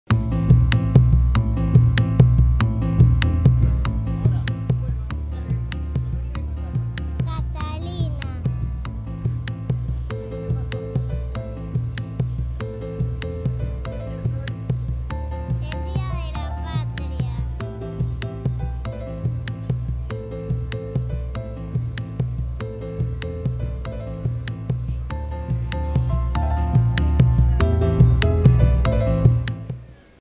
Chicos y chicas festejaron el 25 de mayo
Durante los festejos por el nuevo aniversario de la revolución de mayo la Defensoría del Público llevó a cabo diferentes actividades para empoderar a las audiencias en sus derechos comunicacionales. Más de 500 chicos y chicas se acercaron a la carpa inflable de la Defensoría donde dejaron un mensaje en video relatando su experiencia en los festejos por el aniversario del 25 de mayo. También en conjunto con la Secretaría de Agricultura Familiar, las audiencias participaron de una radio abierta.